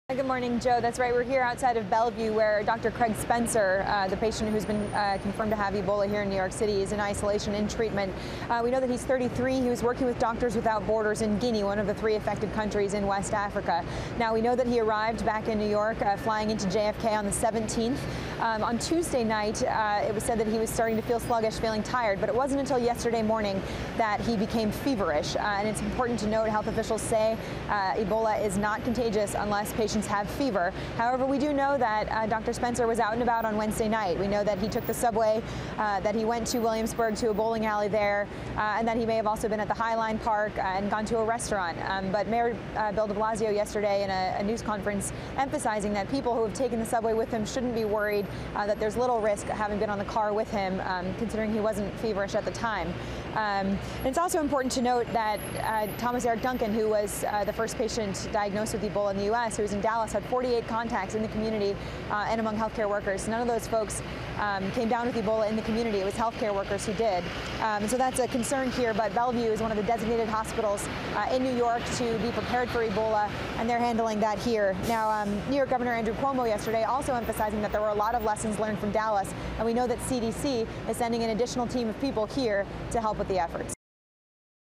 NYC Doctor Tests Positive for Ebola | CNBC
Dr. Craig Spencer has tested positive for the Ebola virus, reports CNBC's Meg Tirrell.